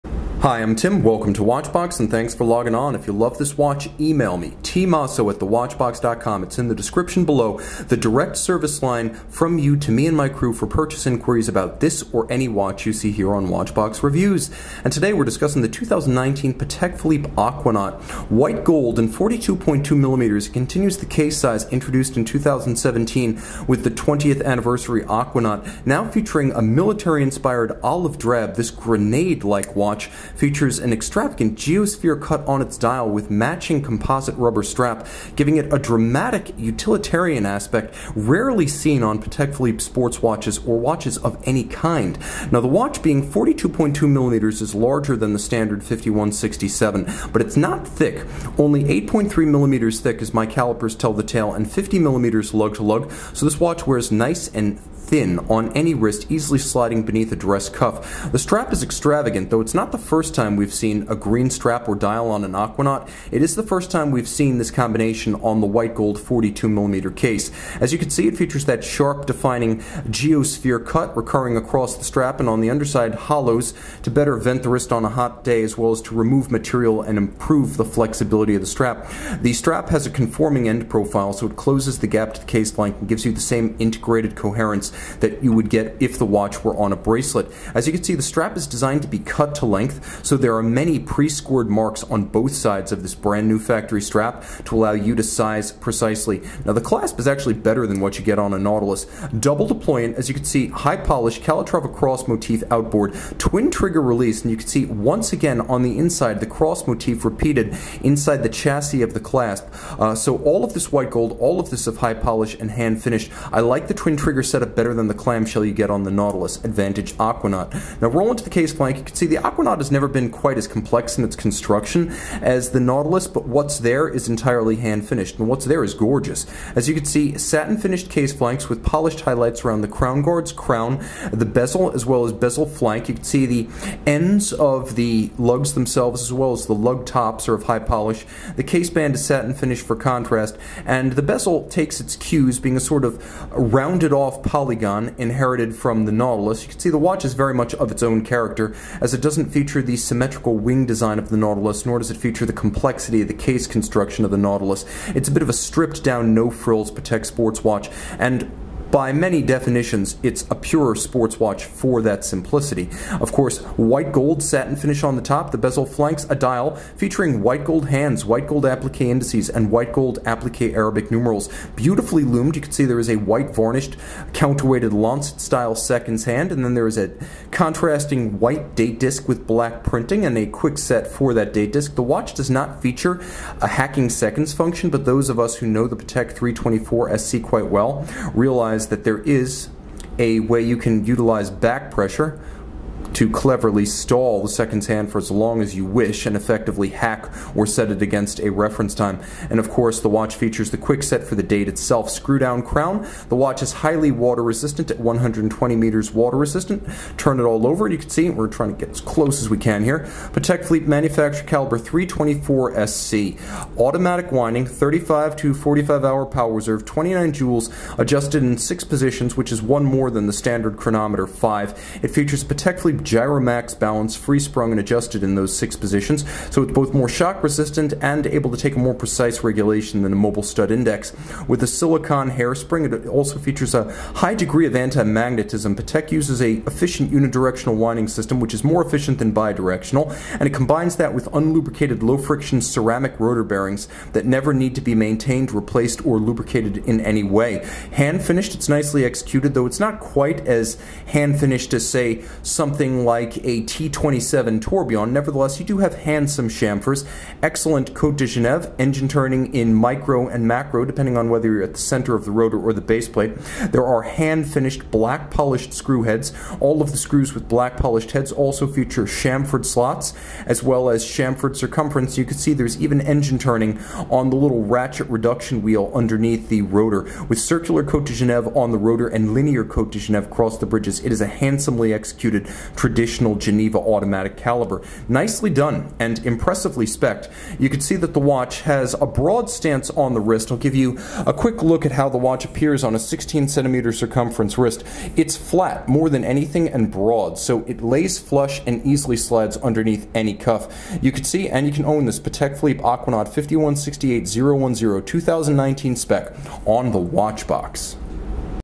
2019 Patek Philippe Aquanaut "Military Green" 5168G-010 Patek Philippe Watch Review
The 2019 Patek Philippe Aquanaut 5168G-010 military/olive green is encased in 42.2mm of white gold surrounding a green dial on a matching rubber strap. Features of this green Patek Philippe Aquanaut include hours, minutes, seconds, and date.

For complete details, watch the full Patek Philippe Aquanaut review by Tim Mosso!